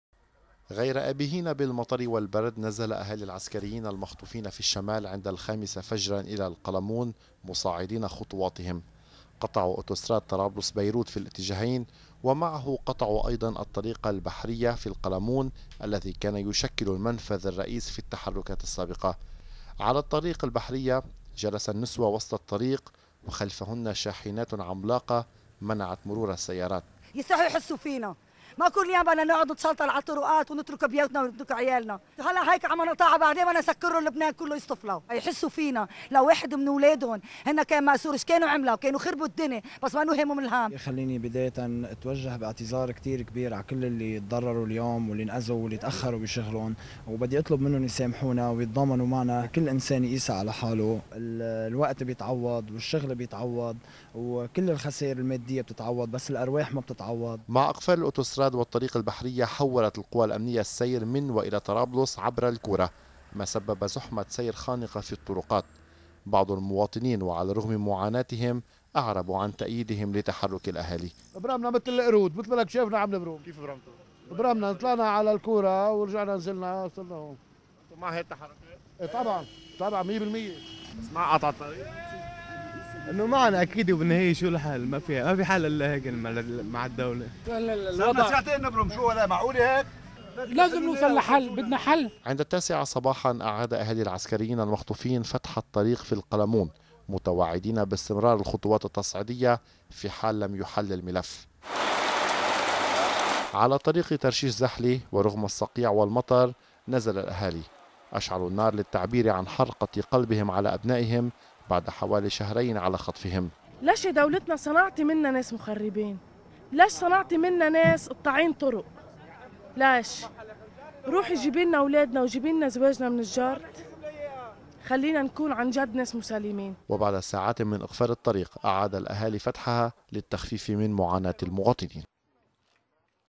LBCI_Newsأهالي العسكريين يواصلون تصعيدهم بين الشمال والبقاع
عمد أهالي العسكريين المختطفين إلى قطع الاتوستدراد الدولي والطريق البحرية في القلمون بالات